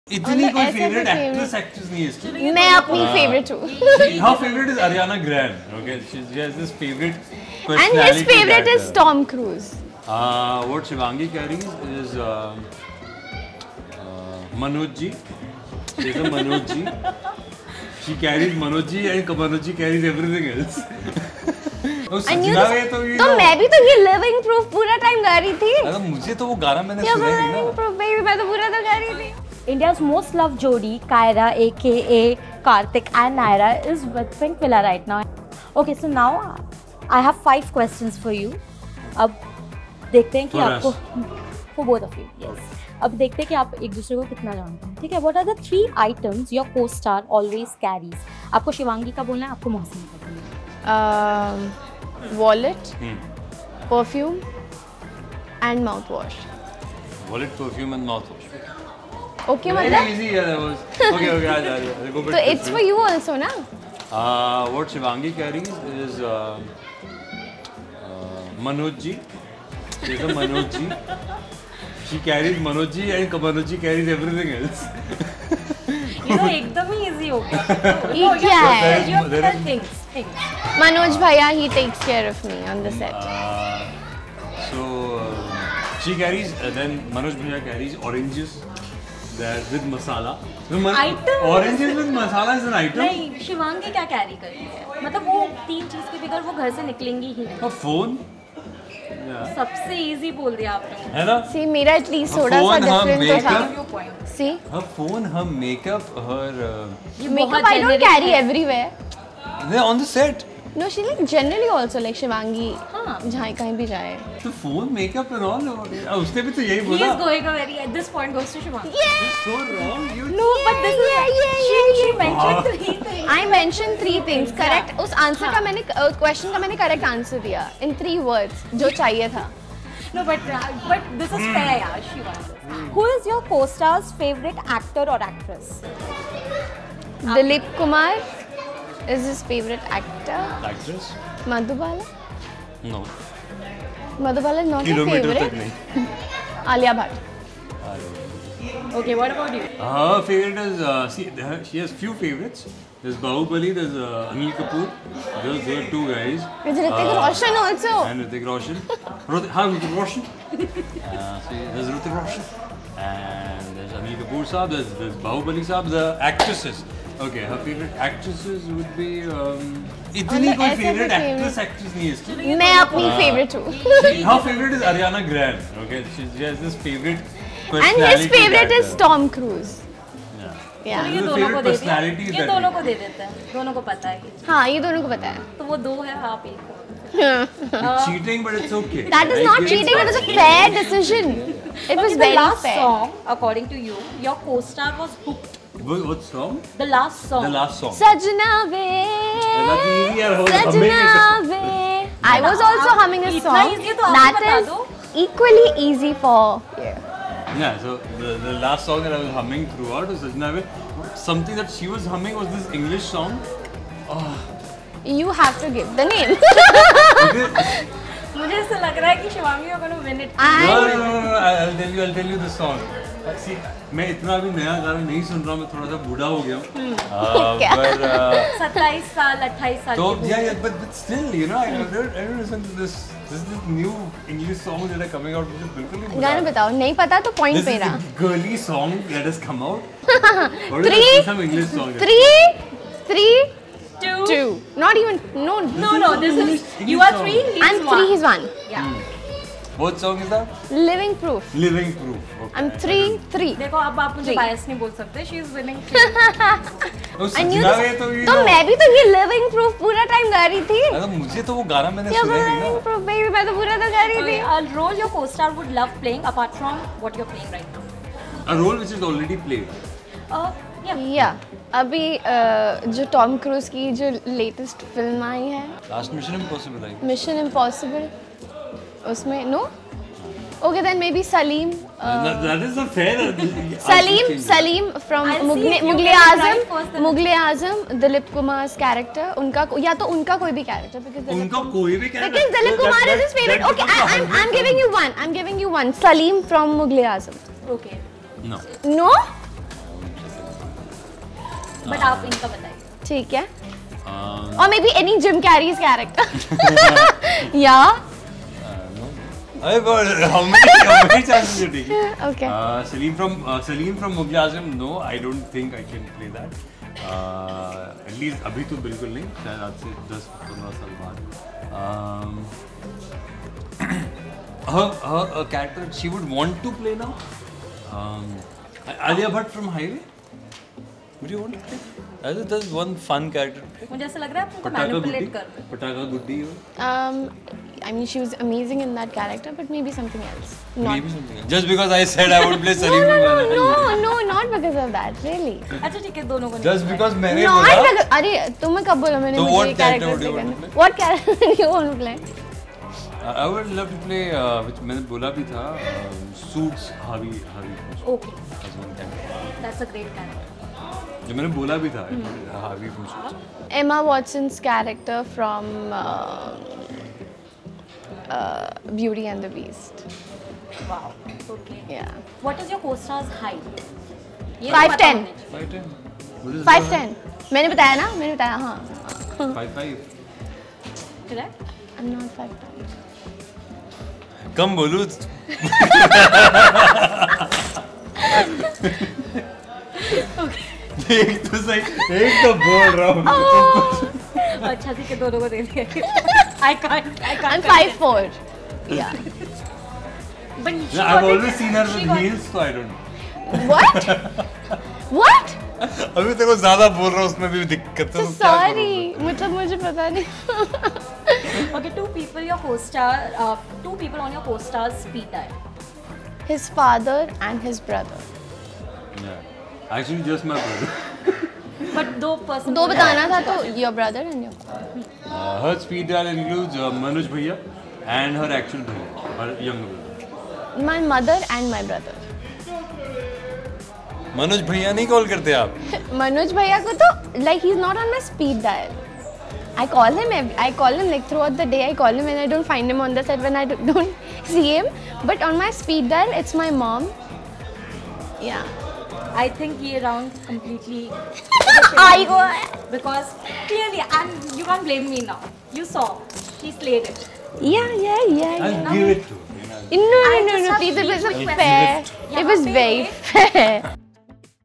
Mohsin Khan and Shivangi Joshi take the compatibility test | Yeh Rishta Kya Kehlata Hai | Kaira
Mohsin Khan and Shivangi Joshi of Yeh Rishta Kya Kehlata Hai fame continue to remain one of the most loved couples. Ending year on a high note, in an exclusive chat with Pinkvilla, the two took the compatibility test. The two were extremely hilarious and funny whilst answering for each other and to know who won between the two, please watch the video.